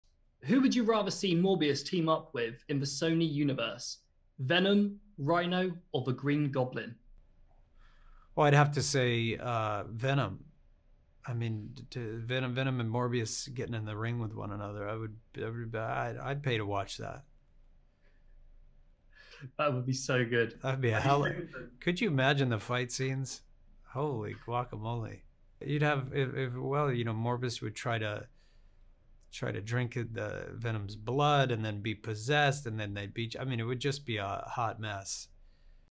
Jared Leto's Plot For Morbius 2: It's Morbin' Time 😂
Could Venom and Morbius team up in a Morbius sequel? Now that really would be Morbin' Time 😂